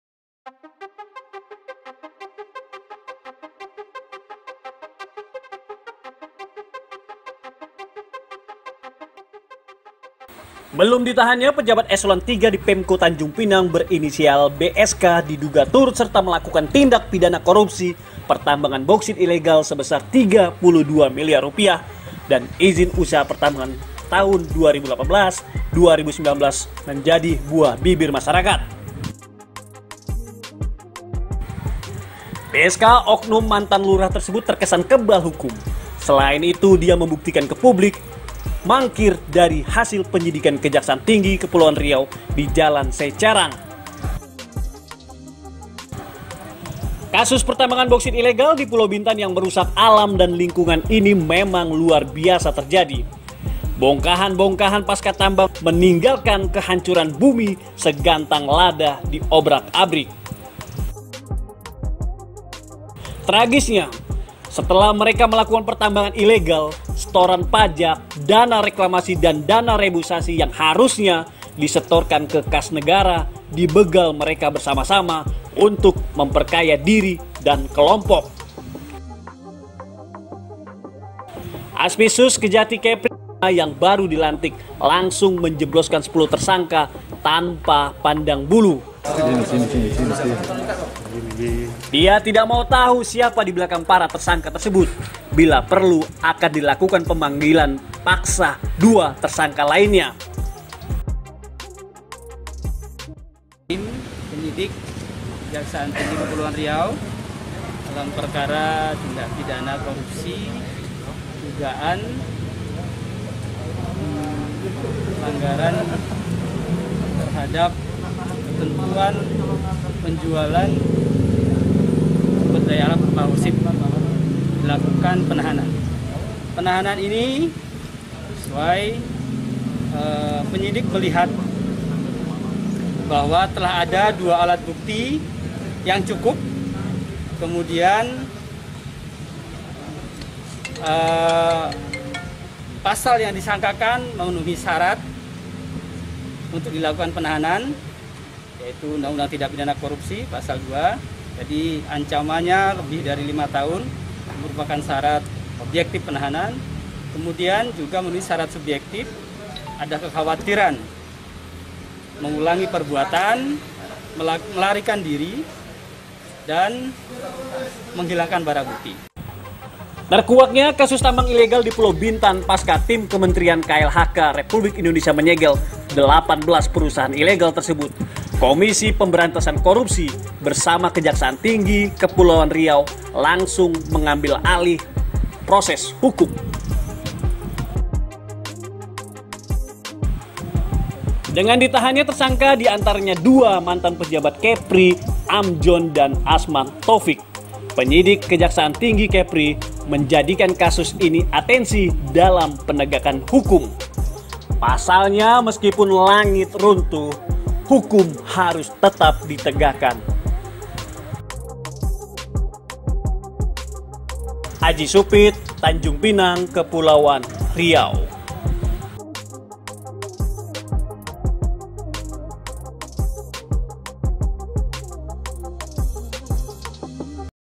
PEJABAT PEMKO TANJUNGPINANG MANGKIR DI PANGGIL KEJATI KEPRI, DIDUGA KORUPSI BAUKSIT
Pejabat Pemko Tanjungpinang tersangka Tindak Pidana Korupsi Bauksit Ilegal di Pulau Bintan Kepulauan Riau Tahun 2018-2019 mangkir dipangil Kejati Kepri. sebelumnya 10 tersangka ditahan dan dijebloskan ke penjara Rutan Tanjungpinang.

Penyidik Kejati Kepri menahan 10 tersangka diantaranya ada dua mantan pejabat kepri  tersebut sudah memenuhi unsur Tindak Pidana Korupsi. selain itu 2 alat bukti cukup dan  takut para tersangka melarikan diri lalu menghilangkan alat bukti.